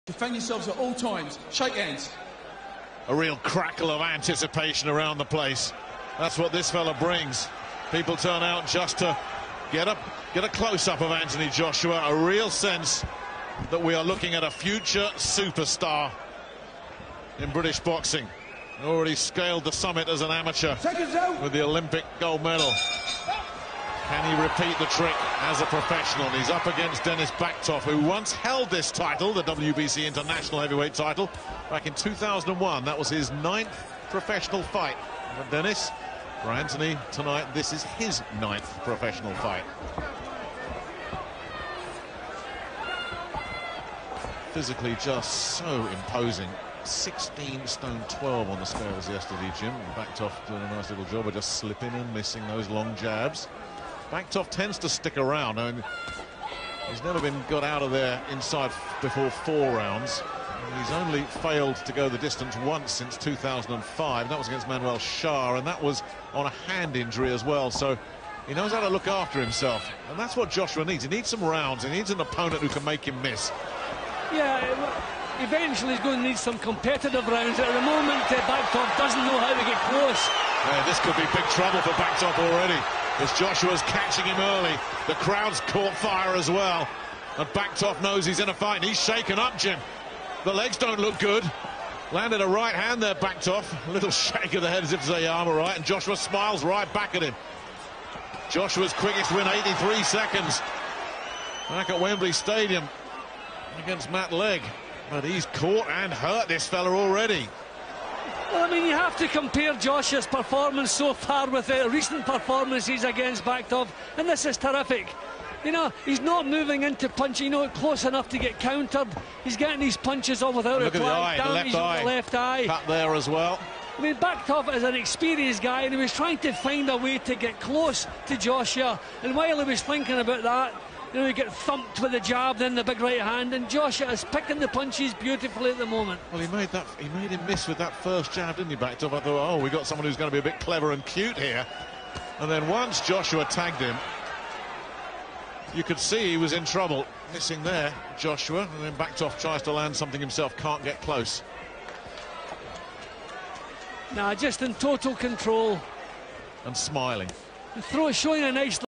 Joshua: Ohara Davies boxes Andy Keates for English lightweight title
Ohara Davies will face Andy Keates for the vacant English lightweight title at The O2 on April 9, live on Sky Sports Box Office.

The bout is part of a huge night of boxing as Anthony Joshua challenges Charles Martin for the IBF world heavyweight belt